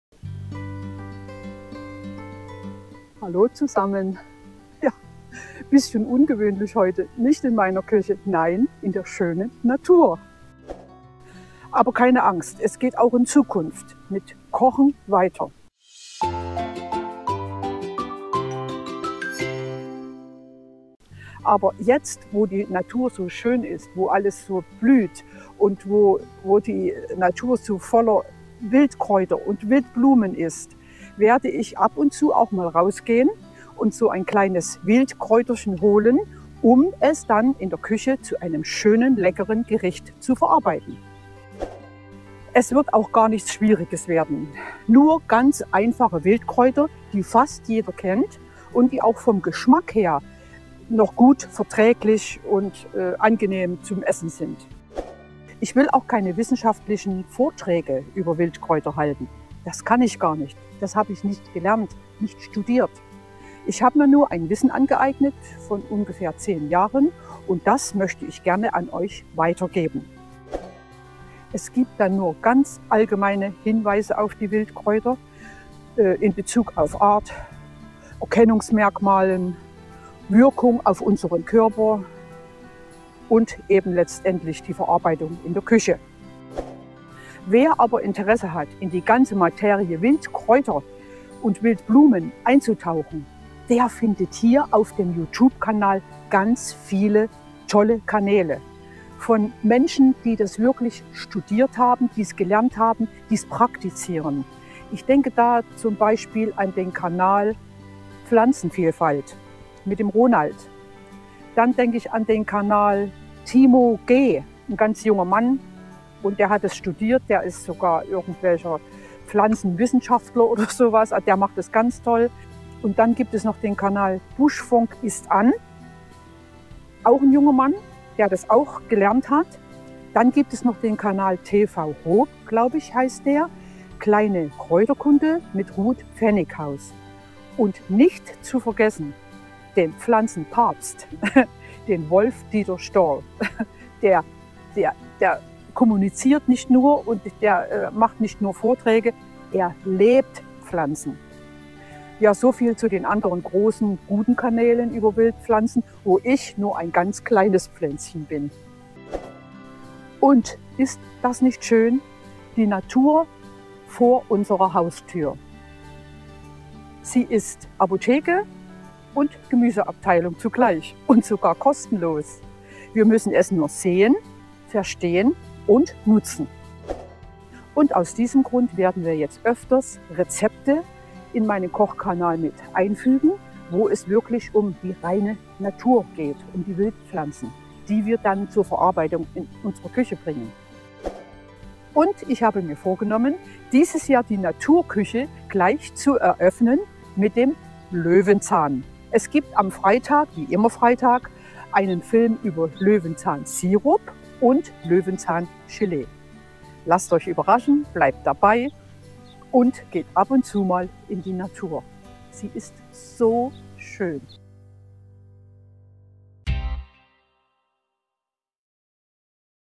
Nur eine Information/ es geht um Wildpflanzen
Ja es soll auch nur eine Information sein, das ich in Zukunft immer mal ein paar Videos von Wildpflanzen und der Verarbeitung  in meiner Küche mit bringen möchte.

* TV Hope, kleine Kräuterkunde mit Ruth Pfennighaus - überwiegend Gesundheit